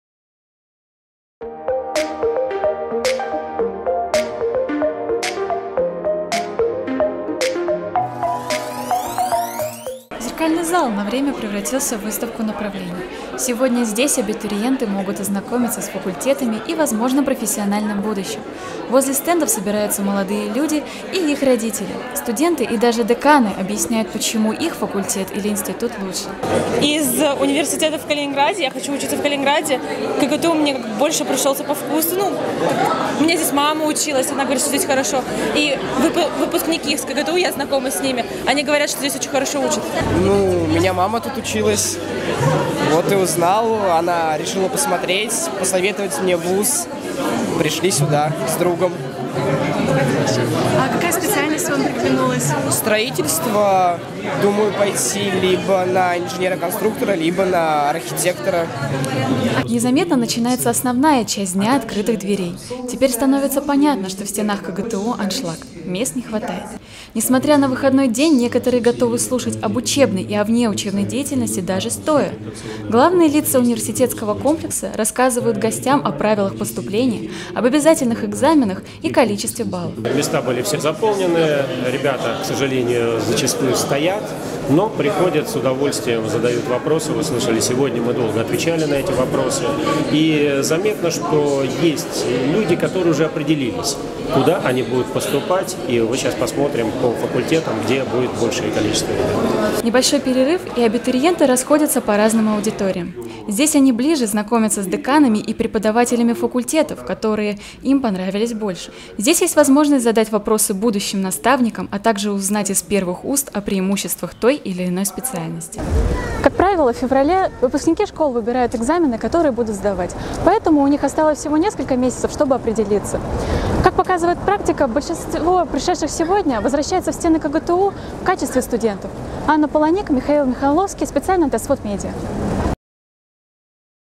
День открытых дверей в КГТУ
В актовом зале КГТУ собрались более 300 человек, желающих узнать все возможности и перспективы, которые может предоставить наш вуз. Причина тому - День открытых дверей.